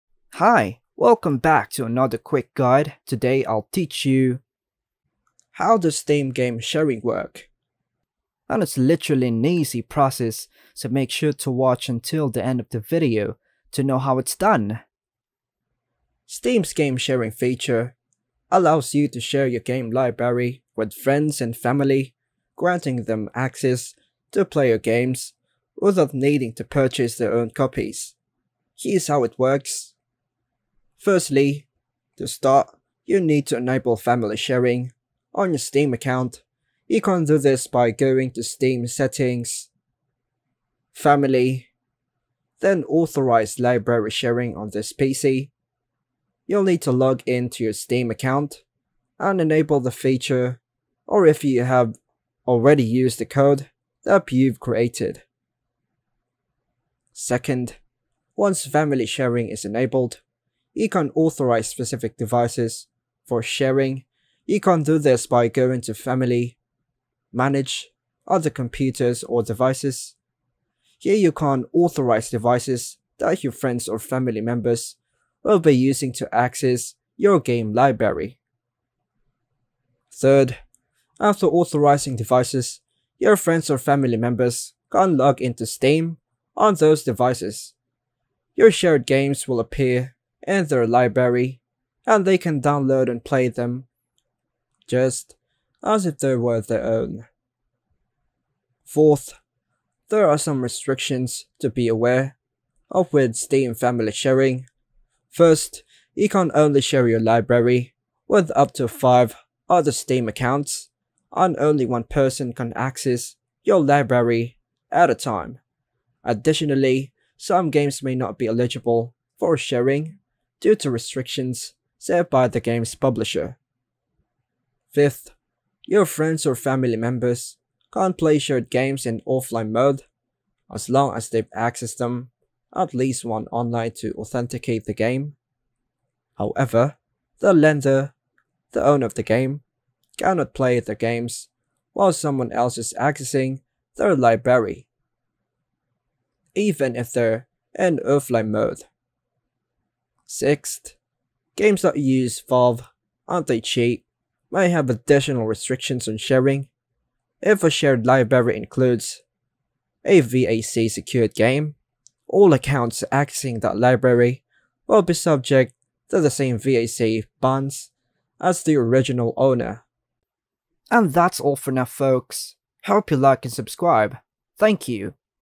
How Does Steam Game Sharing Work (Step-by-Step Guide)
In this video, I will answer, how does Steam game sharing work. This can be a confusing feature at first but after watching, you will have a better understanding of how you can share your Steam games with friends and family with this feature.